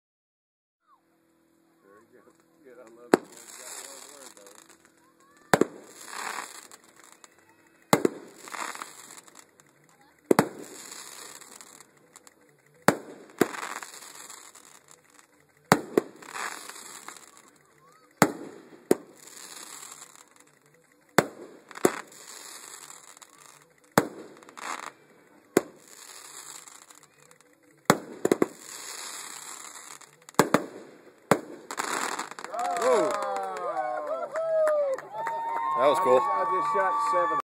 Pecos Fireworks 2012 #11
2012 4th of July fireworks in Pandale, Texas. On the Pandale Crossing bridge.
The end of this one is my favorite.. Much thanks to the two gentlemen who loaned me a chair and 2 Coors Light tallboys!